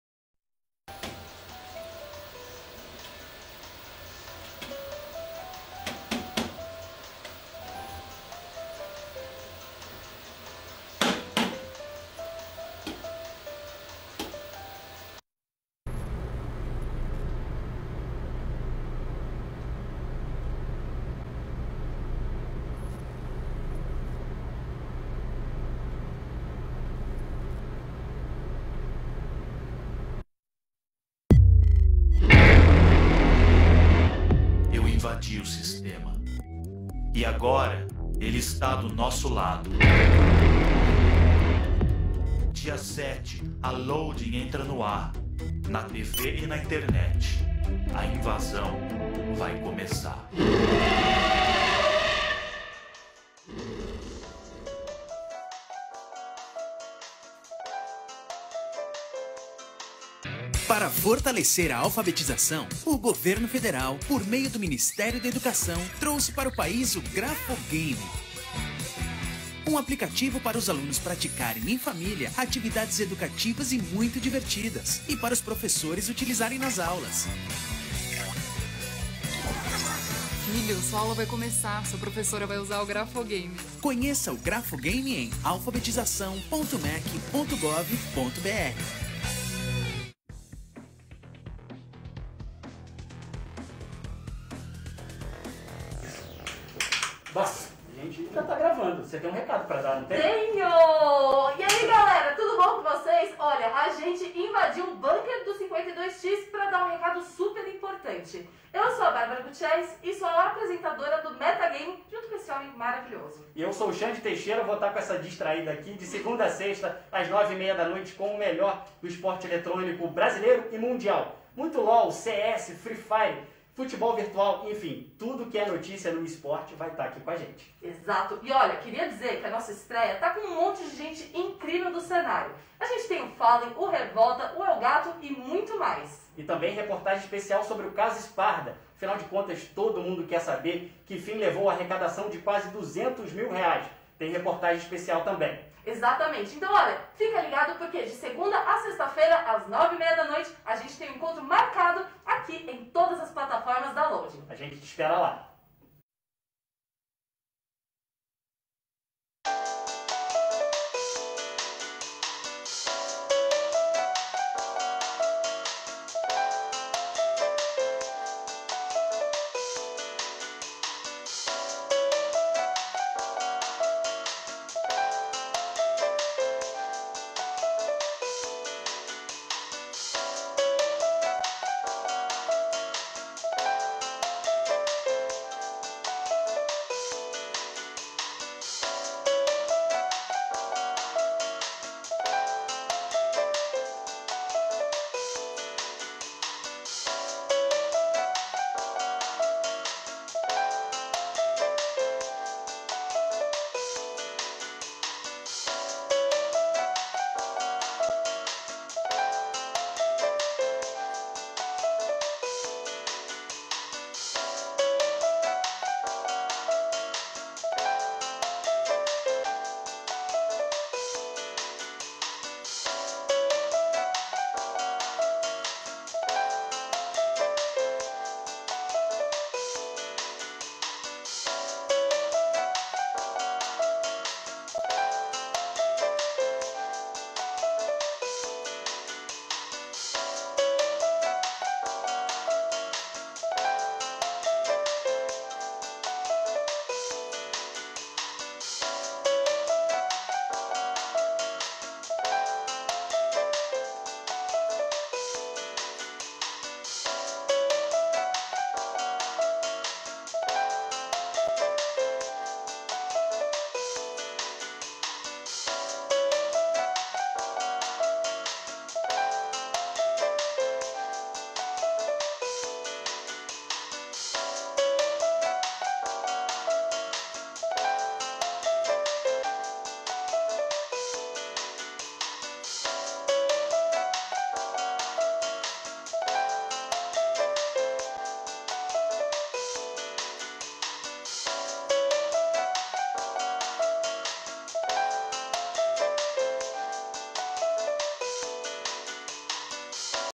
Trecho da programação de expectativa para a estreia da Loading TV (07/12/2020)
Canal 32.1 HD em São Paulo